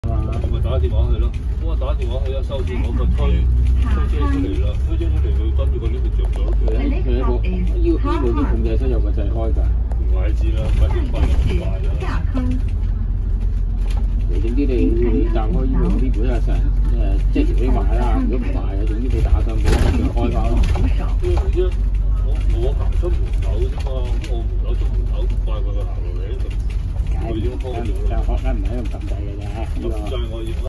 九巴74K線 - 下坑 + 請緊握扶手 報站（RA340）
（粵）下一站係下坑。
（英）The next stop is Ha Hang.
（普）下一站是下坑。